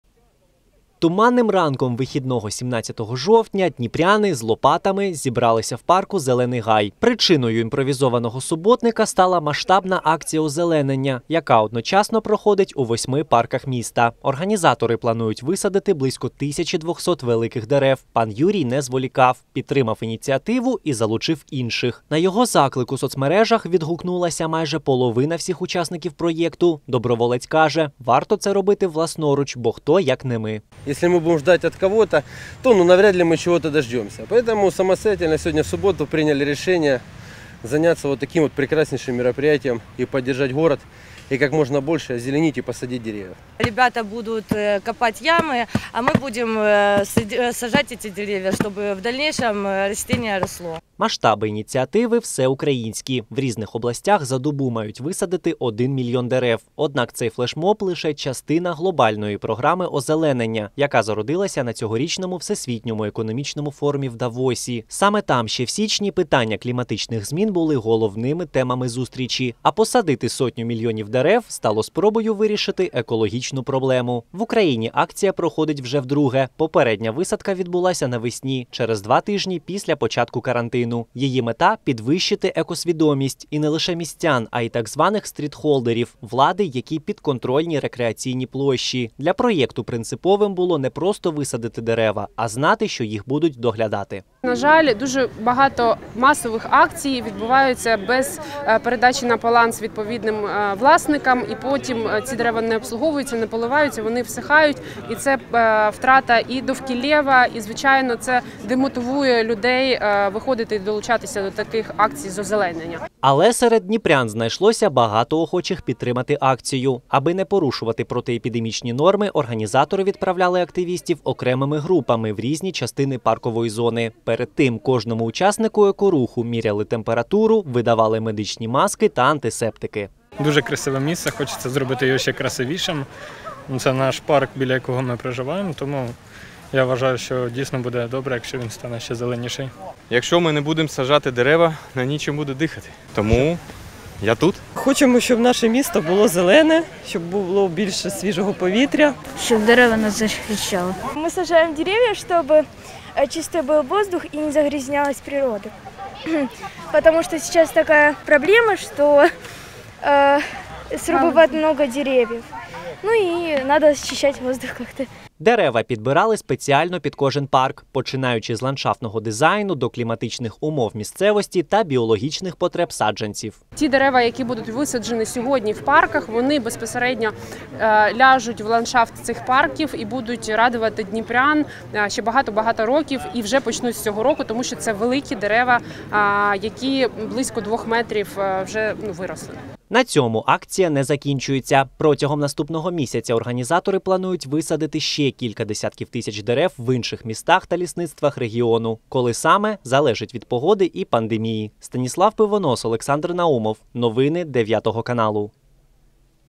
У Дніпрі стартувала друга ітерація проєкту «Мільйон дерев за 24 години»
Озеленення всеукраїнських масштабів: 17 жовтня у Дніпрі вдруге відбувся проєкт «Мільйон дерев за 24 години». Ініціативу реалізовують представники «CEO Club Ukraine» спільно з «Книгою рекордів України». Головна мета акції – відновити екосистему країни та світу в цілому та розвивати екологічну свідомість людей.